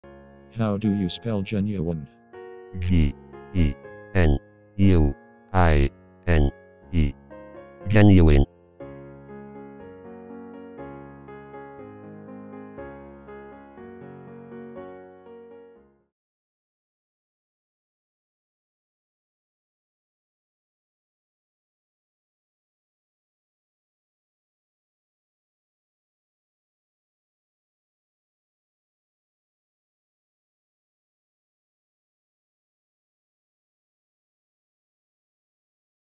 genuine spelling
Learn how to spell and pronounce genuine.